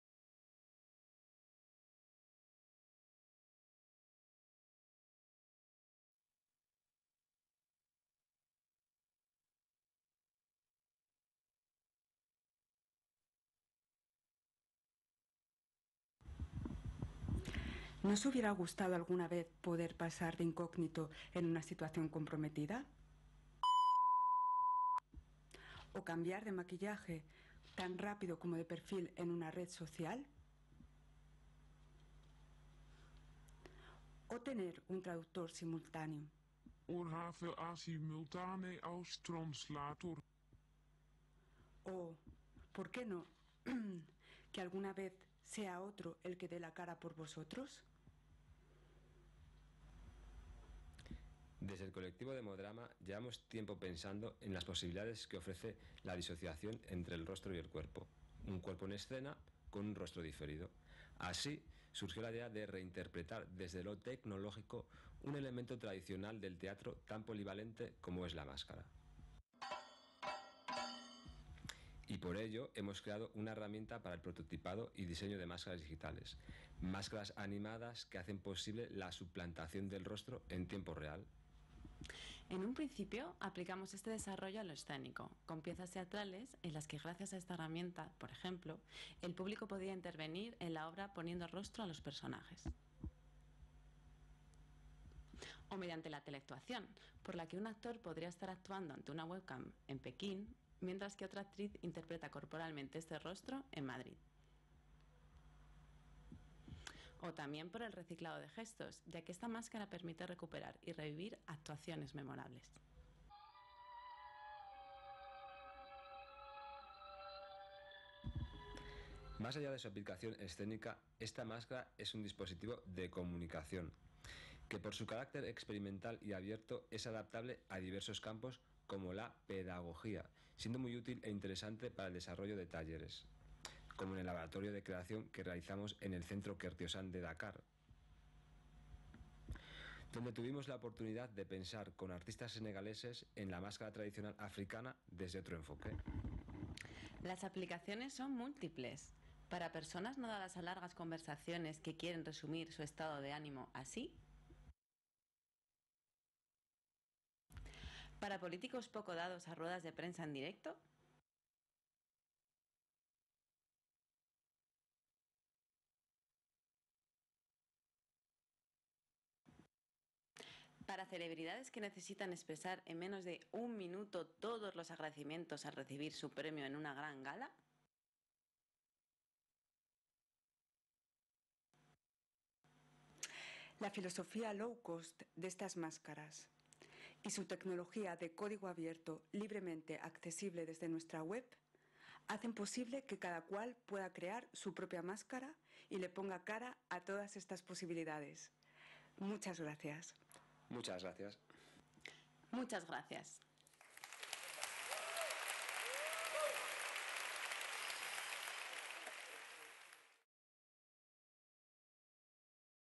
Technology and the mask: Demodrama at TEDxMadrid
The collective of artists Demodrama experiments with the separation between body and face and the intersection between the mask and technology. In this project,  pre-recorded or live images can be projected on the white screen of the mask, allowing endless creative possibilities. Plus, they made this low cost technology available for everybody for free.